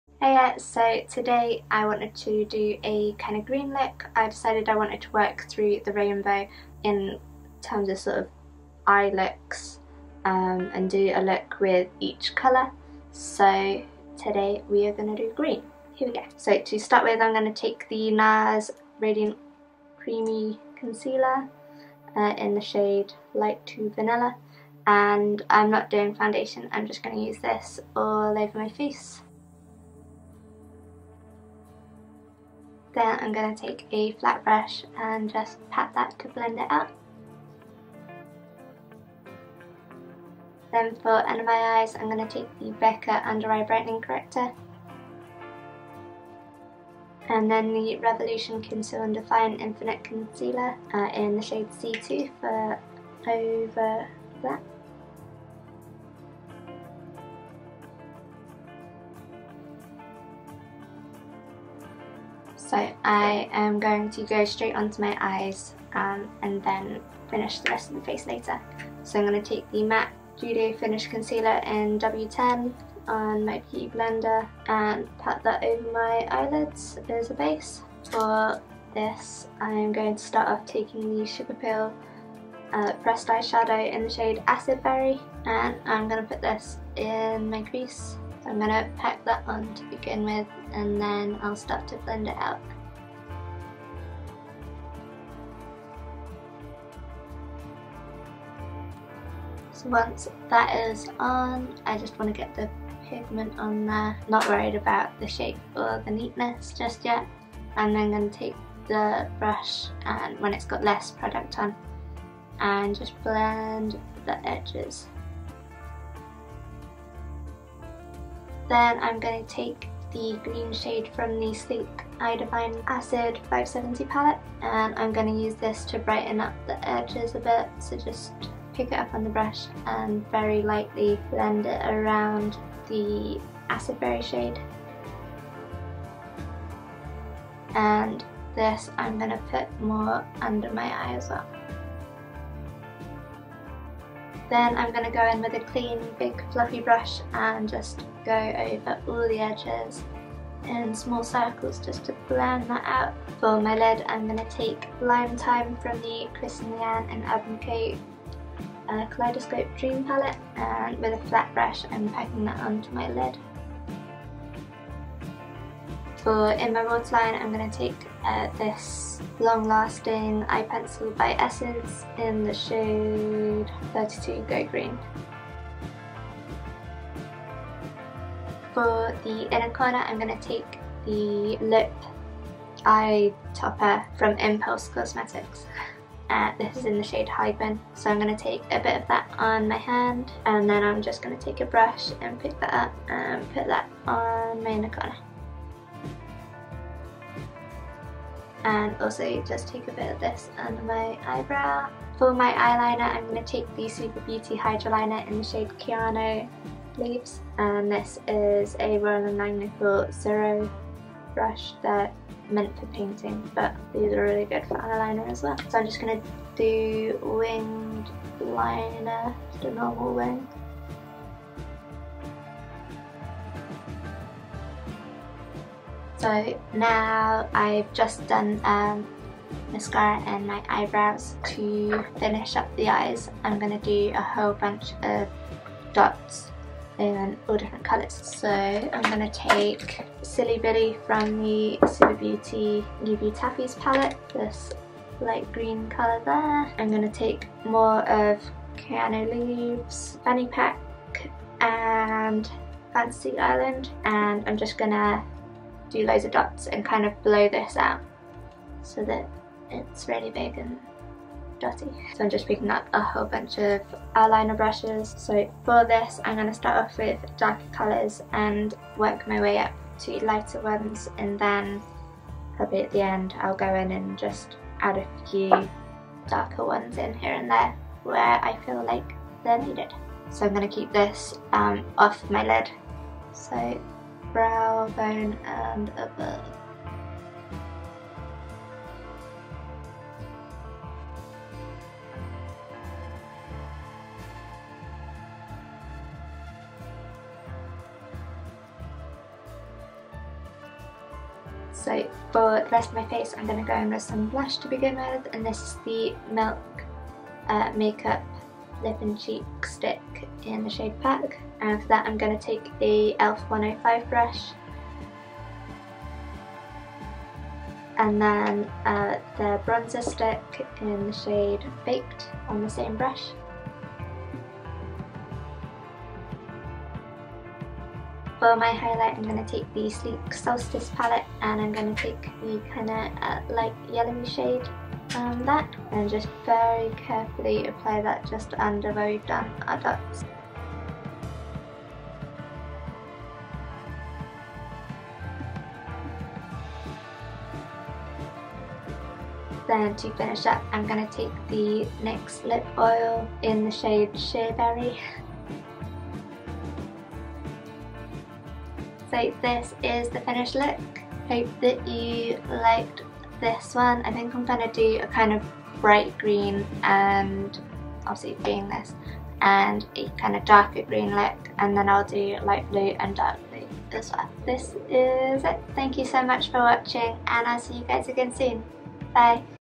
green- monochromatic looks
see this look and more on my instagram @mymakeupcaitlin

Products
Concealer - nars cosmetics, becca cosmetics, revolution
Bronzer-Milk makeup
Blush- milk makeup
Highlight- sleek makeup
Eye base- Mac cosmetics
Eyeshadow- sugarpill cosmetics, sleek cosmetics, urban decay, impulse cosmetics
Eyeliner- Suva beauty, essence cosmetics
Lipstick- Nyx professional makeup